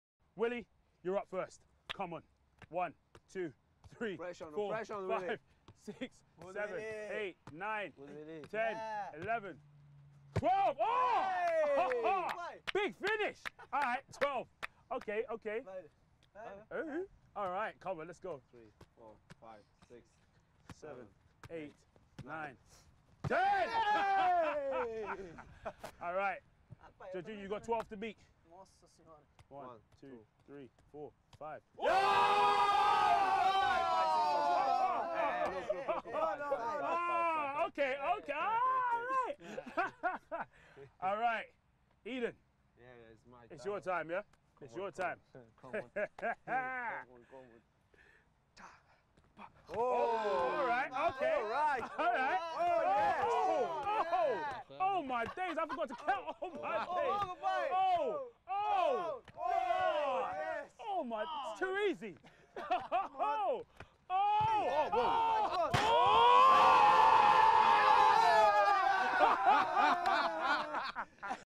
Eden Hazard displays his tekkers with an American Football .... A Thing of beauty!
Chelsea's top player Eden Hazard shows his incredible skills with an American football in this amazing show.  Absolute scenes!